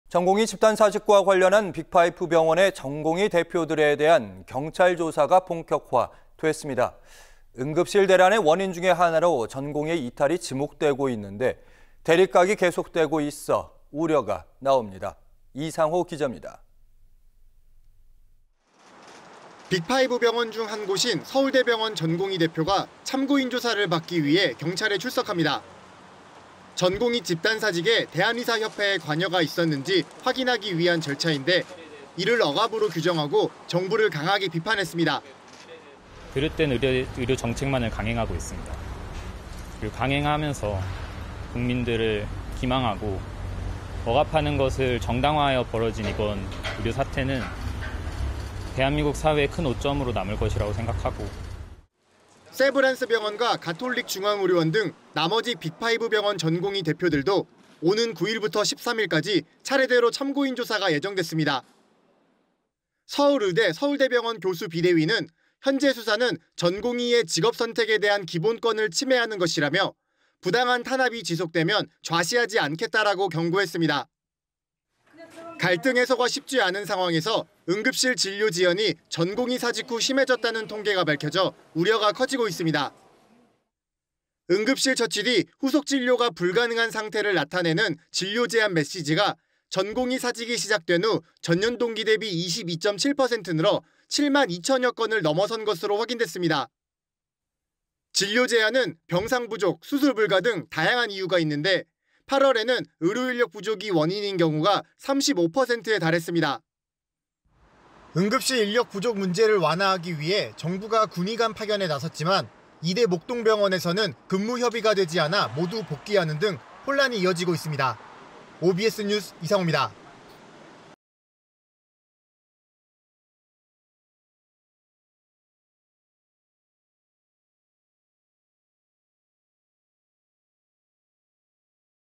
빅5 병원 전공의 대표들… 경찰 조사 본격화..대립각 우려
【앵커】
전공의 집단사직과 관련한 빅5 병원 전공의 대표들에 대한경찰 조사가 본격화 됐습니다.
응급실 대란의 원인 중 하나로 전공의 이탈이 지목되고 있는데 대립각이 계속되고 있어 우려가 나옵니다.
 이상호 기자입니다.

【기자】
빅5 병원 중 한 곳인 서울대병원 전공의 대표가 참고인 조사를 받기 위해 경찰에 출석합니다.

전공의 집단사직에 대한의사협회의 관여가 있었는지 확인하기 위한 절차인데 이를 억압으로 규정하고 정부를 강하게 비판했습니다.

[박재일 / 서울대병원 전공의 대표: (정부는) 그릇된 의료정책만을 강행하고 있습니다. 강행하면서 국민들을 기망하고 억압하는 것을 정당화하여 벌어진 이번 의료사태는

대한민국 사회의 큰 오점으로 남을 것이라 생각하고….]

세브란스 병원과 가톨릭중앙의료원 등 나머지 빅5 병원 전공의 대표들도 오는 9일부터 13일까지 차례대로 참고인 조사가 예정됐습니다.

서울의대·서울대병원 교수 비대위는 "현재 수사는 전공의의 직업 선택에 대한 기본권을 침해하는 것"이라며 "부당한 탄압이 지속되면 좌시하지 않겠다"라고 경고했습니다.

갈등 해소가 쉽지 않은 상황에서 응급실 진료 지연이 전공의 사직 후 심해졌다는 통계가 밝혀져 우려가 커지고 있습니다.

응급실 처치 뒤 후속 진료가 불가능한 상태를 나타내는 '진료제한' 메시지가 전공의 사직이 시작된 후 전년 동기 대비 22.7% 늘어 7만2천여 건을 넘어선 것으로 확인됐습니다.

진료제한은 병상부족, 수술 불가 등 다양한 이유가 있는데 8월에는 의료인력 부족이 원인인 경우가 35%에 달했습니다.

응급실에서 근무하던 전공의와 일반의 90% 이상이 이탈하며 생긴 공백을 단기간에 메우기는 쉽지 않아 비상진료는 계속될 수밖에 없어 보입니다.

OBS뉴스 이상호입니다.